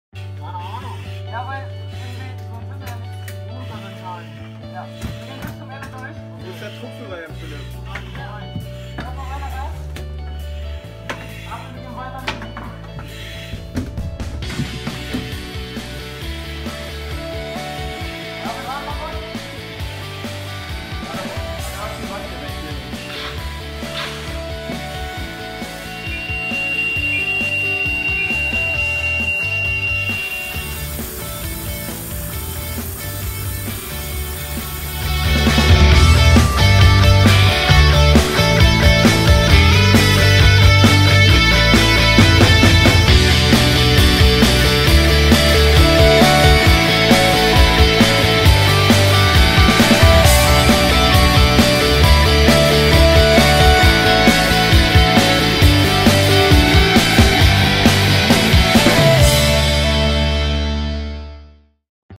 Unter Atemschutz im Kindergarten Echte
Kindergärten sind für Atemschutzgeräteträger ein wahrer "Albtraum". Viele Ecken, kleine Räume und viel Platz für Kinder, um sich bei einem Feuer aus Angst zu verstecken. Aus diesem Grund hat die Feuerwehr Echte den Kindergarten einmal unter Atemschutz besucht – und viel über die Räumlichleiten gelernt.